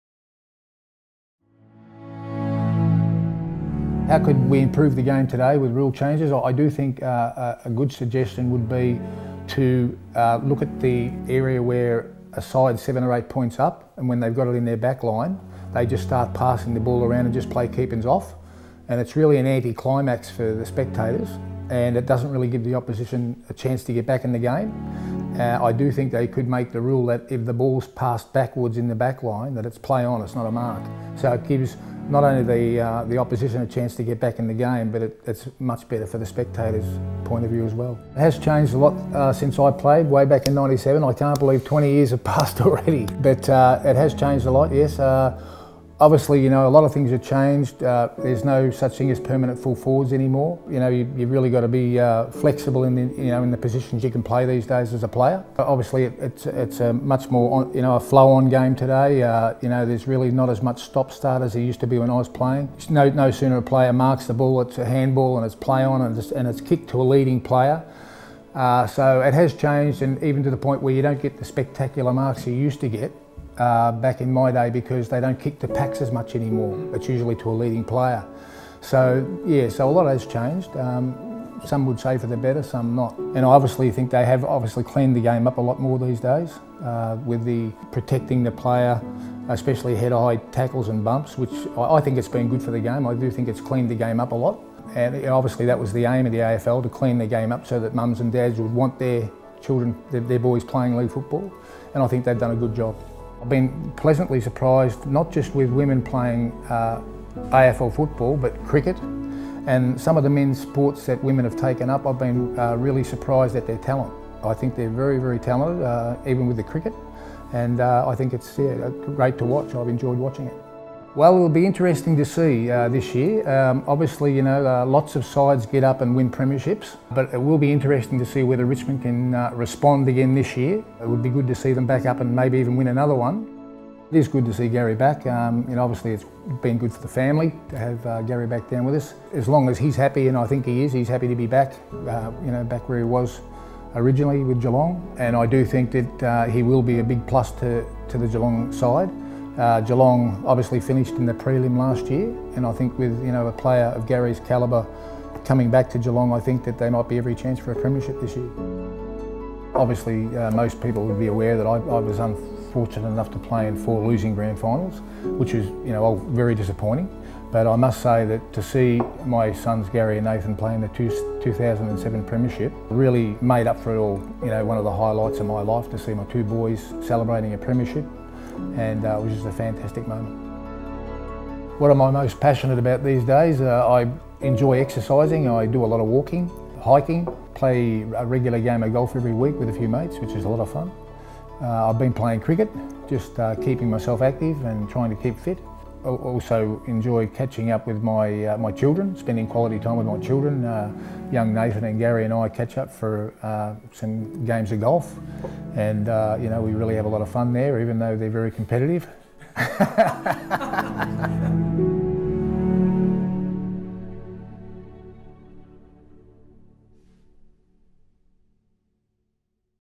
"Where Are They Now" with Gary Ablett Snr presented by Exclusive Insight
In his first candid interview in nearly 20 years, Gary Ablett Snr sits down with Exclusive Insight for the "Where Are They Now" series.
Widely regarded as one of the greatest players of all time, Ablett looks back on his prolific footy career - sharing some of his favourite memories in the game.